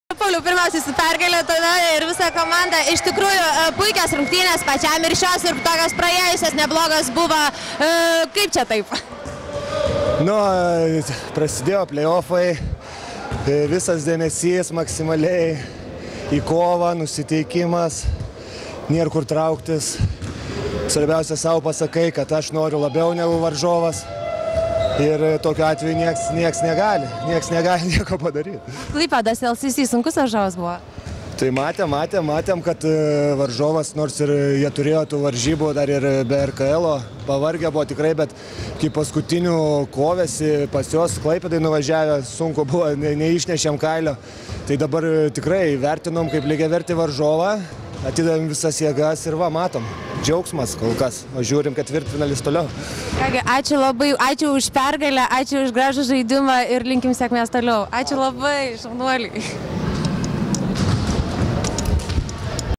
Tauragės KK žaidėjas K.Abramavičius po patekimo į pusfinalį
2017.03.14 Tauragėje buvo žaidžiamos trečiosios Natural Pharmaceuticals-RKL A diviziono ketvirtfinalio rungtynės tarp Tauragės krepšinio klubo ir Klaipėdos LCC tarptautinio universiteto. Seriją iki 2 pergalių laimėjo Tauragės ekipa, varžovus iš uostamiesčio įveikusi rezultatu 72:66.